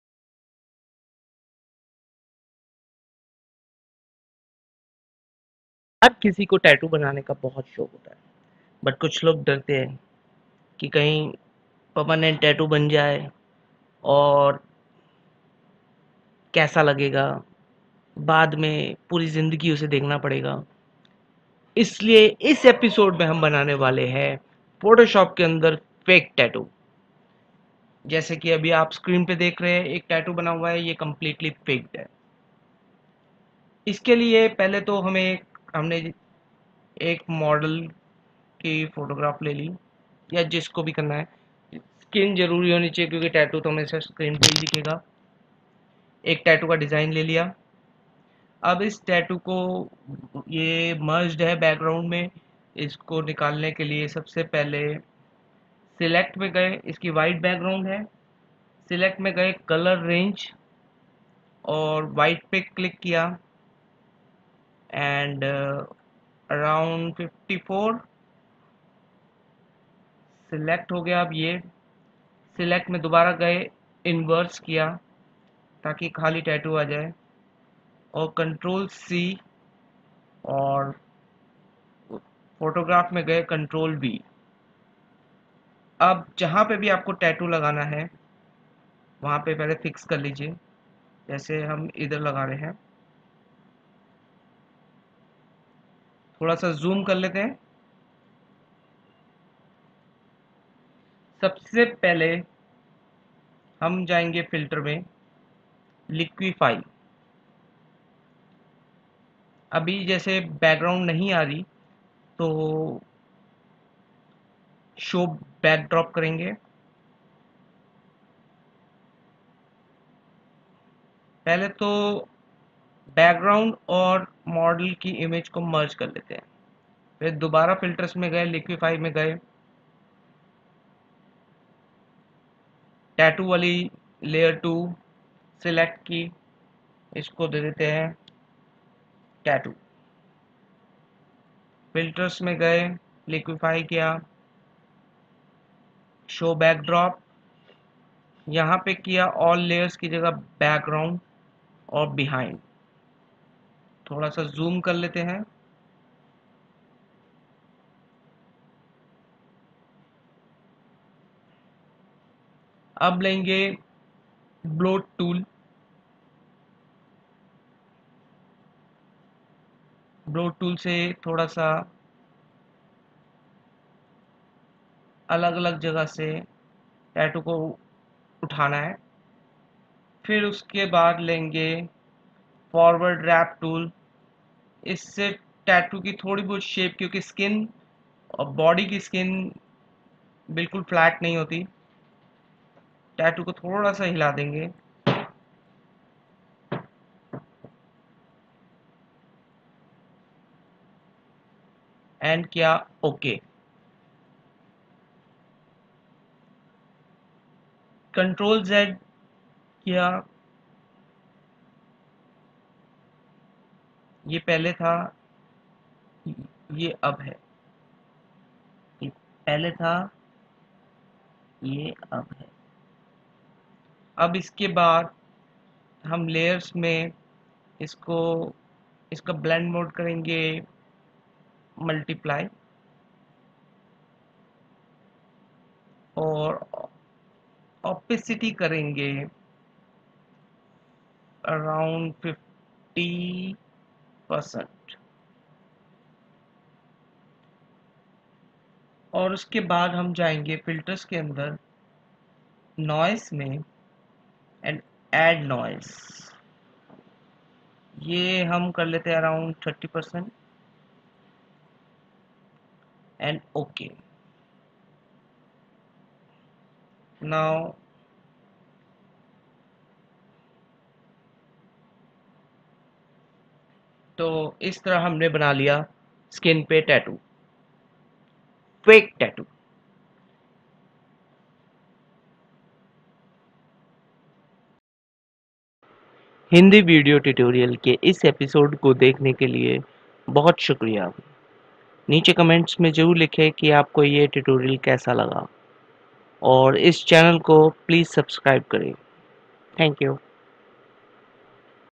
How to Make a Fake Tattoo in Photoshop
Learn how to create a fake tattoo in Adobe Photoshop. This tutorial is in Hindi Language.